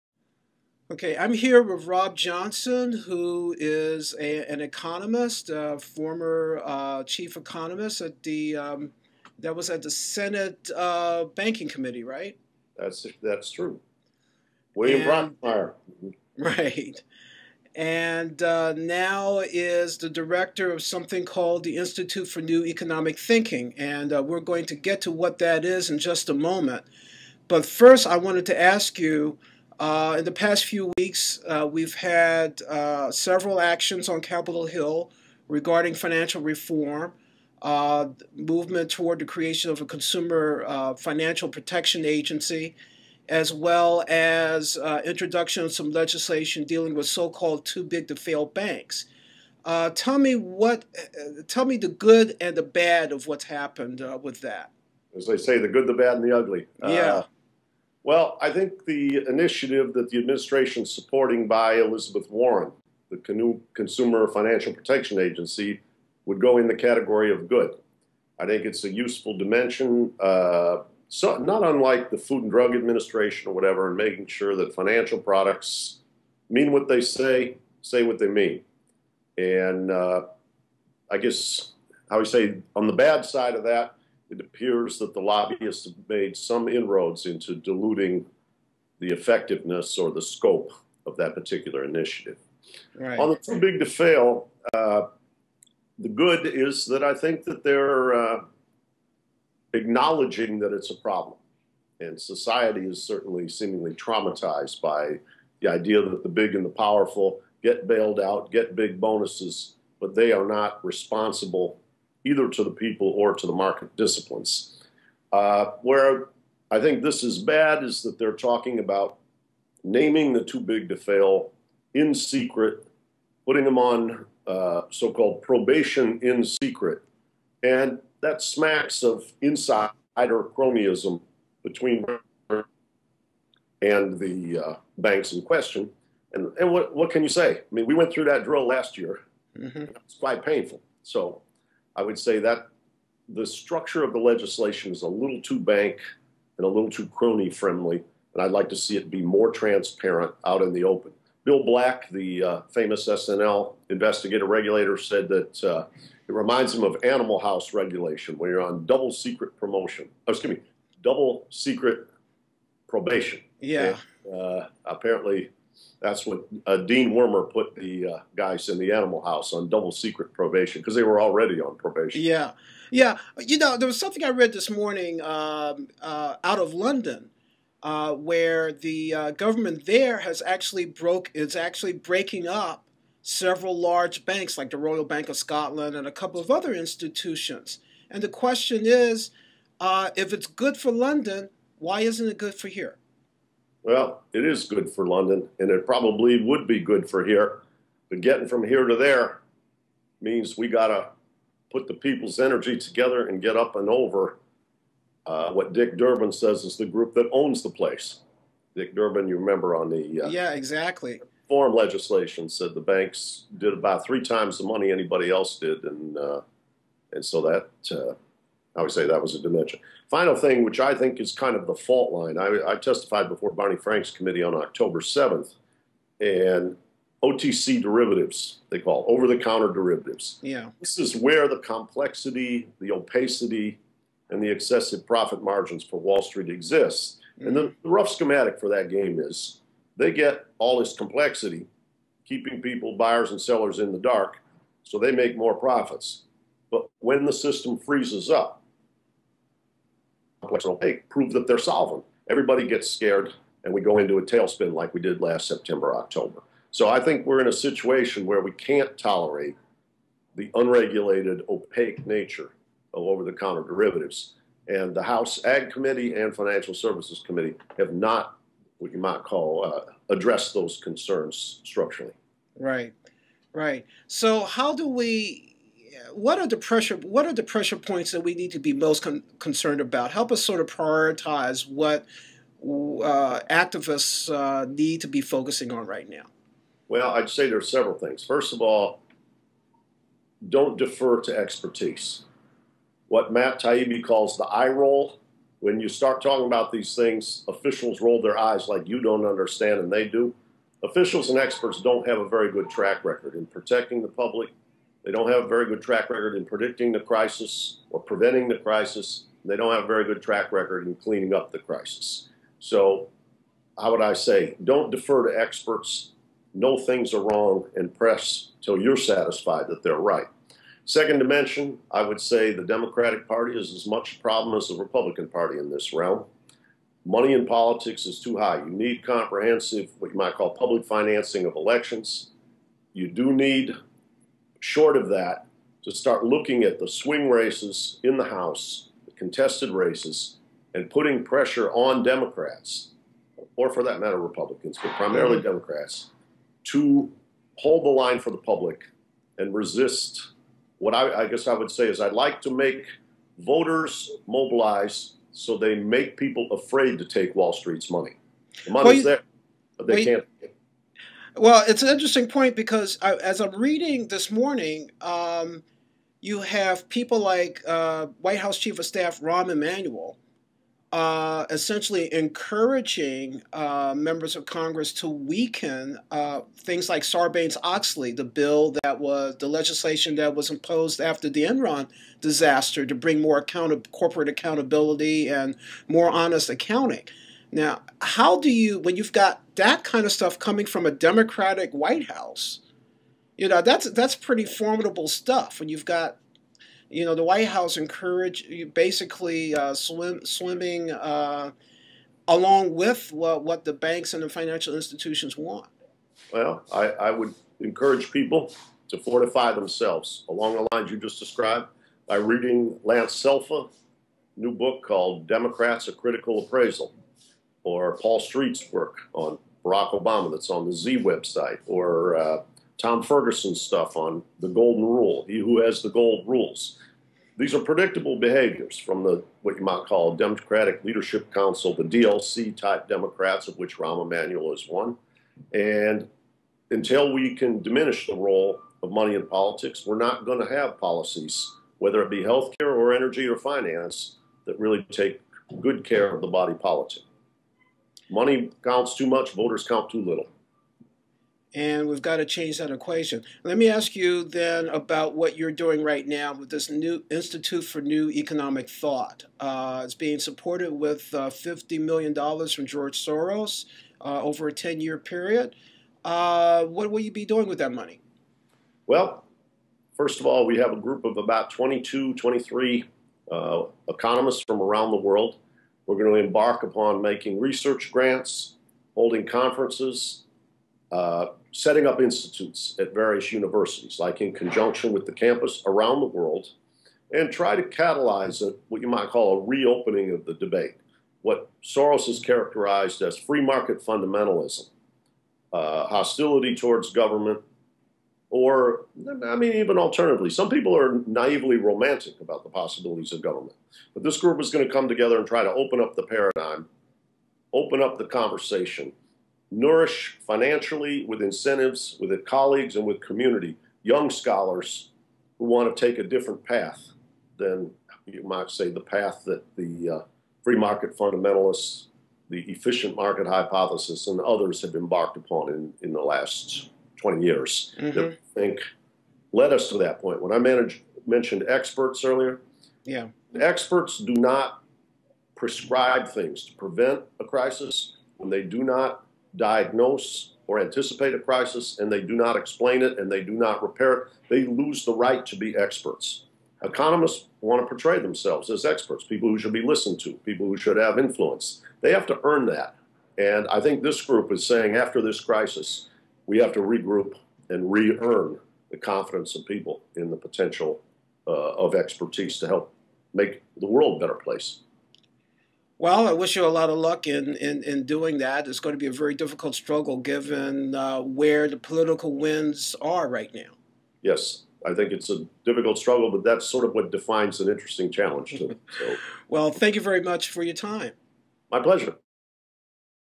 Rob Johnson: Make Politicians Afraid Of Wall Street Money
interviews economist Robert Johnson as he assesses the pros and cons of recent efforts by the White House and Congress to reform the nation's financial markets. Johnson says that the influence of financial industry money in the political process has seriously undermined meaningful financial reform. Johnson is director of the newly created Institute for New Economic Thinking, and he explains how the new organization—backed by a 10-year, $50 million grant by George Soros—hopes to shape the long-term response to today's financial crisis. Interview via Skype by Isaiah J. Poole.